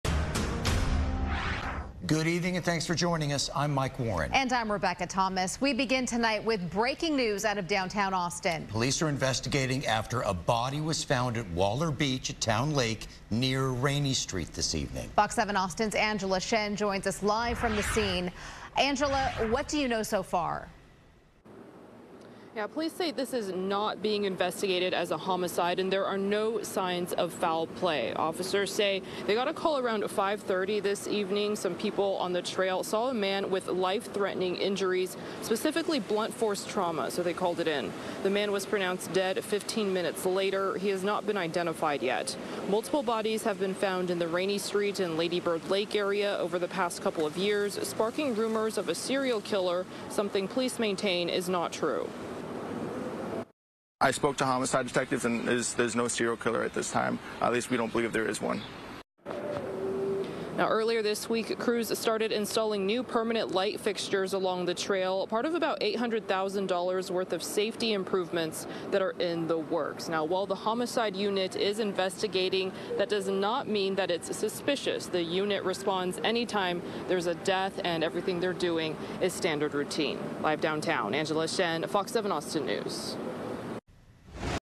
Body found near Rainey Street in Austin, police investigating | FOX 7 Austin
Austin police are investigating after a body was found at Waller Beach at Townlake near Rainey Street.